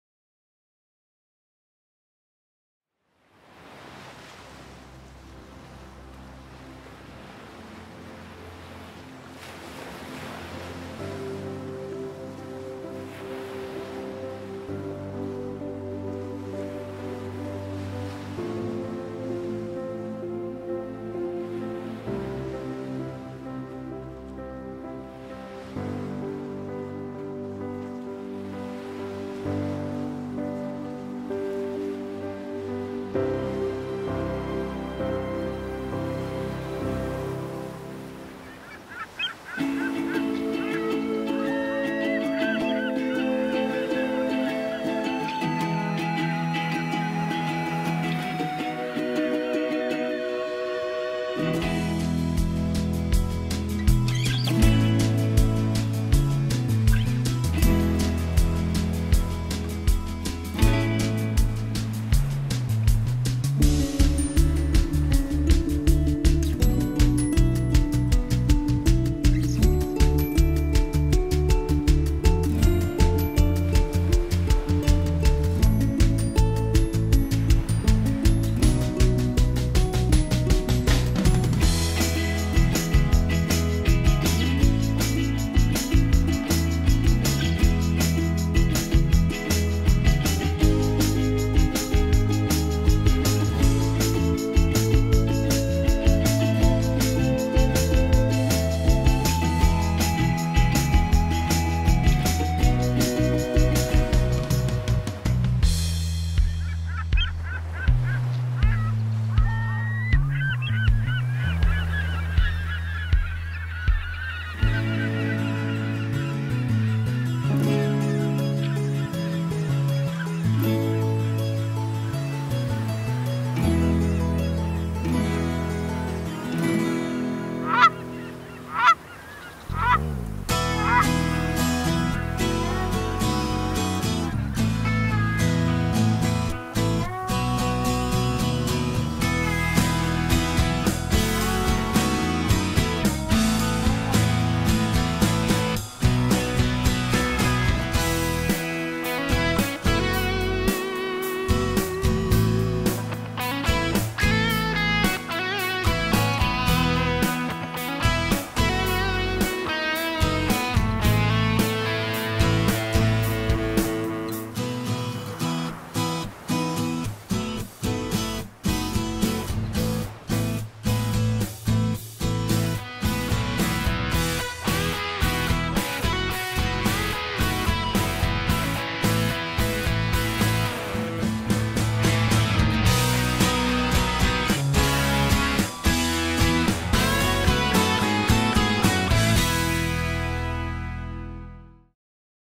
Spring Island 2014
Images from our 2011 and 2013 tours to Spring Island, (off the west coast of Vancouver Island, BC,Canada), announcing our 2014 tour, to be held Aug 13 to Aug 18, 2014.